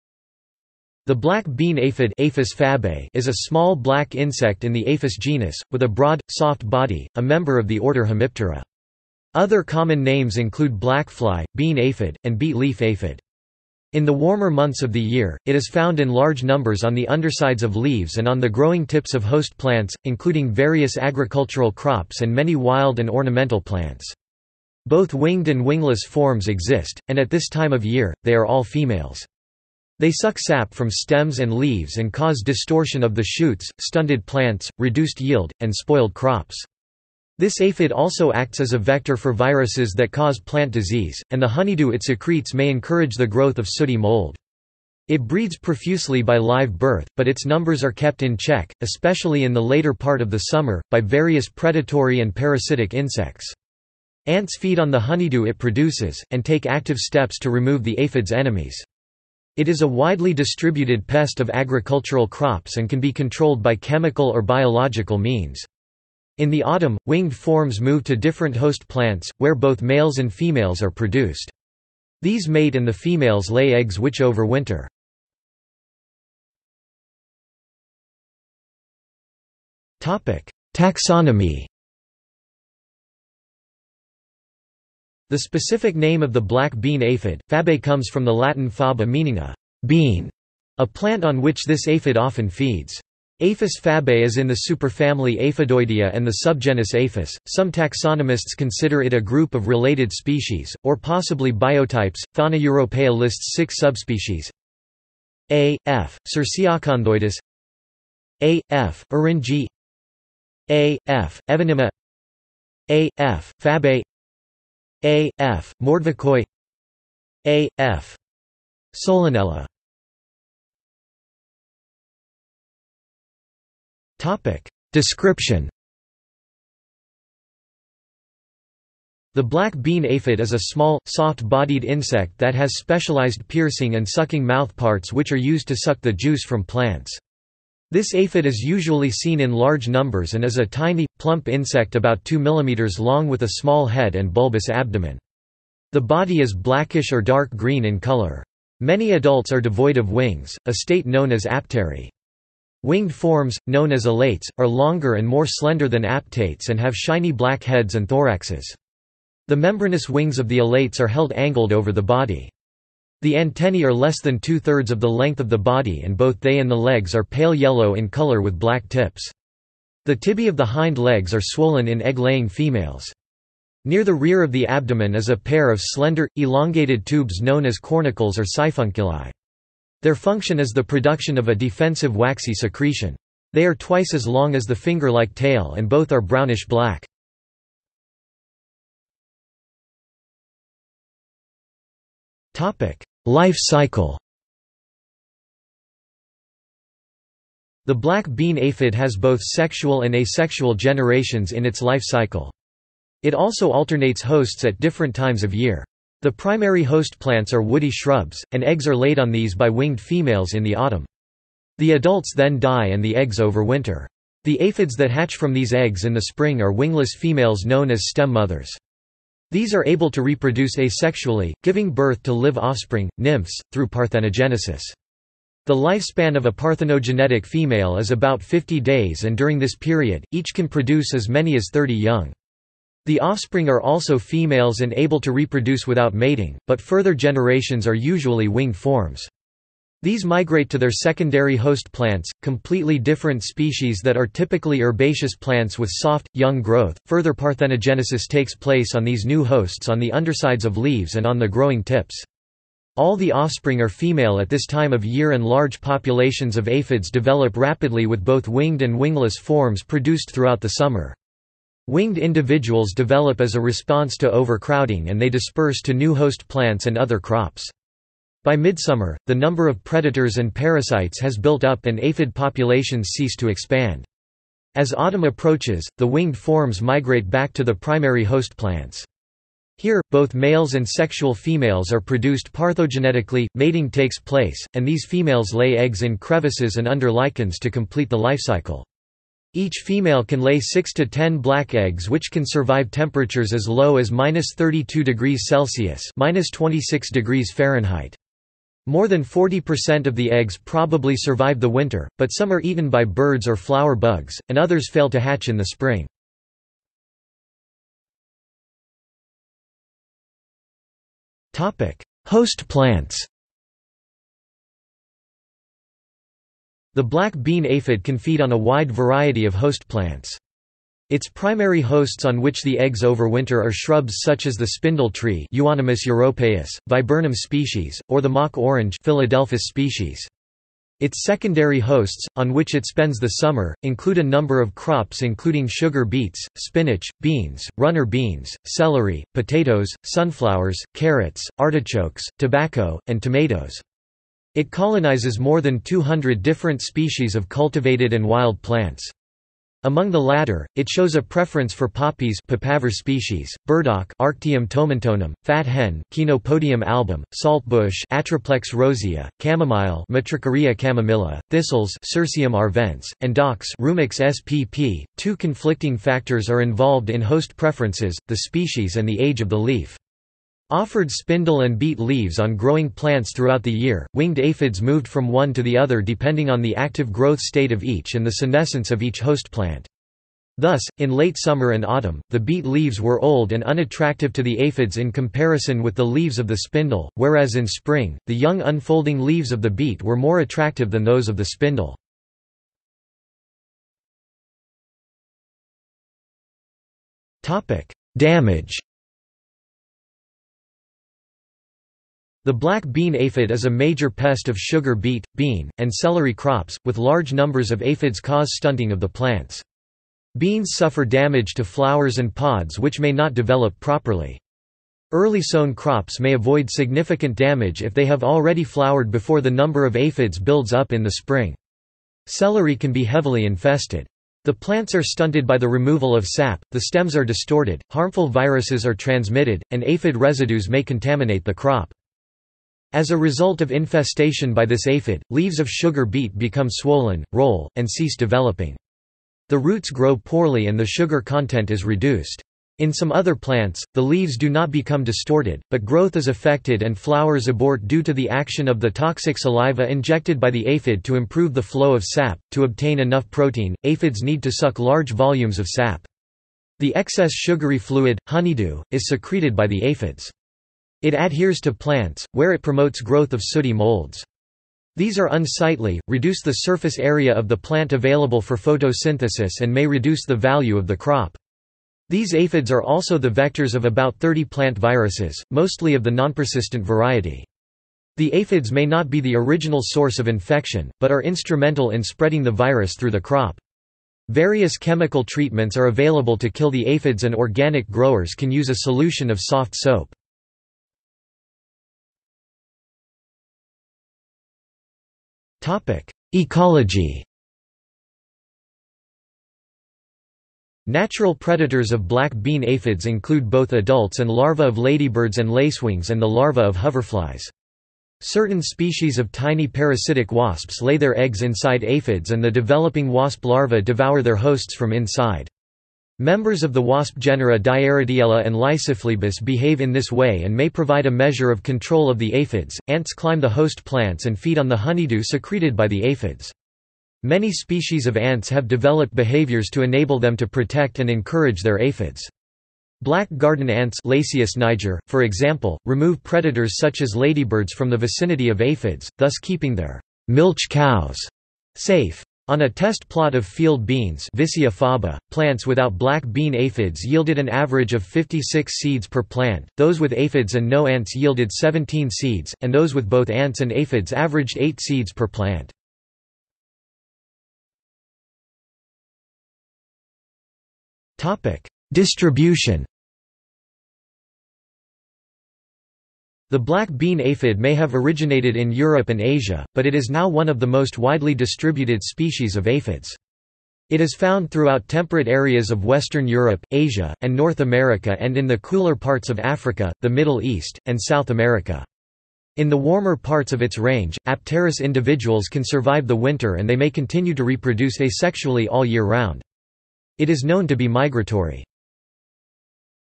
Black bean aphid | Wikipedia audio article
This is an audio version of the Wikipedia Article:
Black bean aphid

00:01:16 1 Taxonomy
00:02:00 2 Description
00:03:08 3 Life cycle
00:05:07 4 Host plants
00:06:33 5 Damage
00:08:12 6 Ecology
00:09:17 7 Distribution

- Socrates

SUMMARY
The black bean aphid (Aphis fabae) is a small black insect in the Aphis genus, with a broad, soft body, a member of the order Hemiptera. Other common names include blackfly, bean aphid, and beet leaf aphid. In the warmer months of the year, it is found in large numbers on the undersides of leaves and on the growing tips of host plants, including various agricultural crops and many wild and ornamental plants. Both winged and wingless forms exist, and at this time of year, they are all females. They suck sap from stems and leaves and cause distortion of the shoots, stunted plants, reduced yield, and spoiled crops. This aphid also acts as a vector for viruses that cause plant disease, and the honeydew it secretes may encourage the growth of sooty mould. It breeds profusely by live birth, but its numbers are kept in check, especially in the later part of the summer, by various predatory and parasitic insects. Ants feed on the honeydew it produces, and take active steps to remove the aphid's enemies. It is a widely distributed pest of agricultural crops and can be controlled by chemical or biological means. In the autumn, winged forms move to different host plants, where both males and females are produced. These mate and the females lay eggs which overwinter.